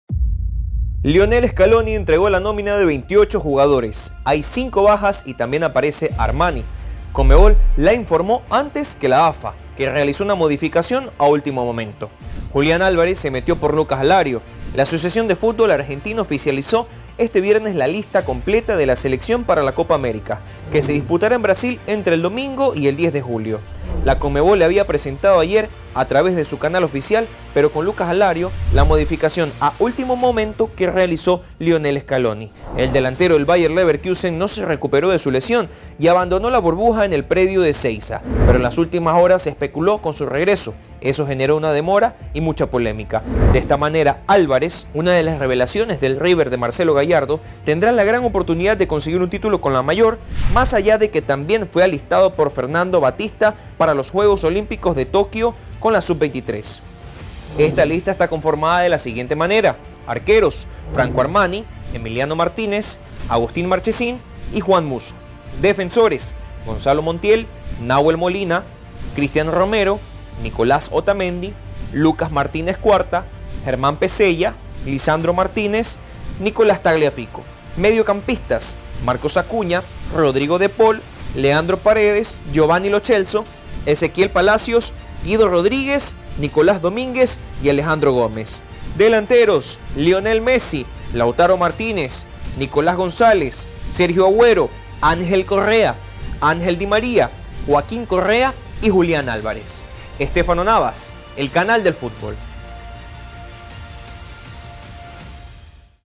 Los convocados de Argentina - Copa América
Lionel Scaloni dio a conocer la lista de 28 convocados para la CopaAmerica